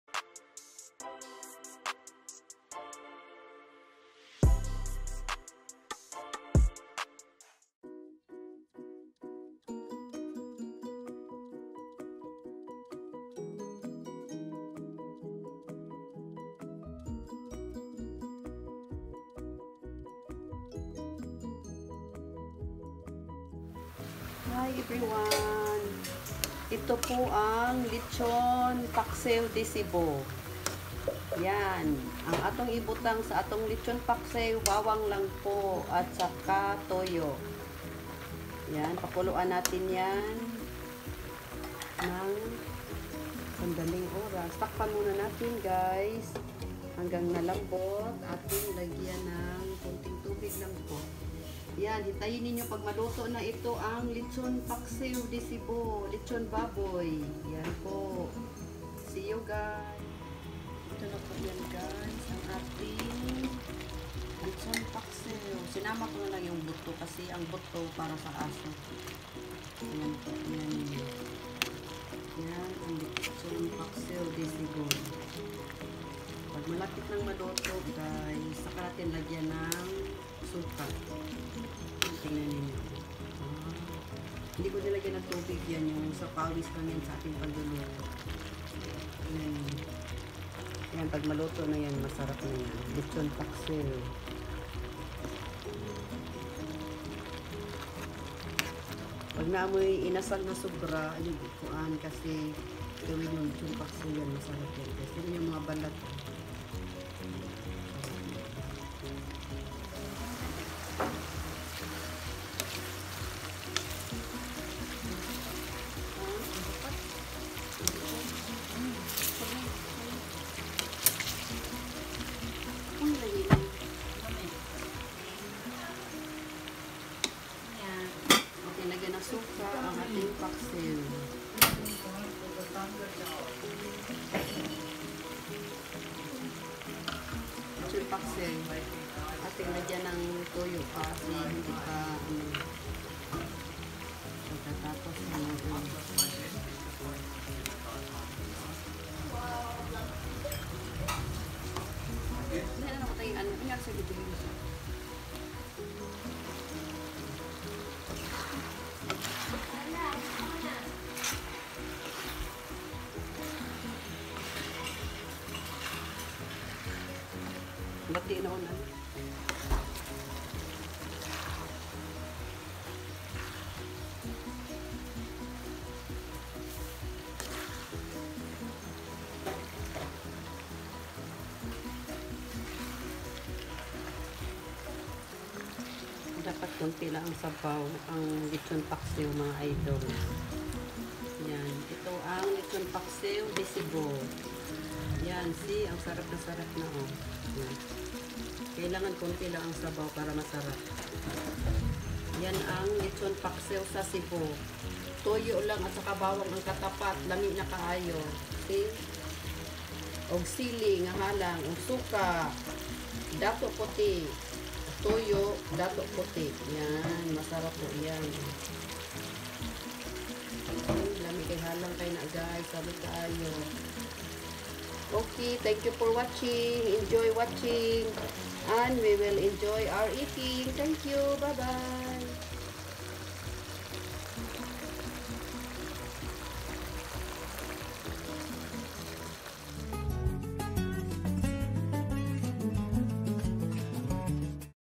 Lechon Paksiw de Cebu
Lechon Paksiw de Cebu lami ka ayo try nyo.

* bawang
* suka datu puti
* tuyo datu puti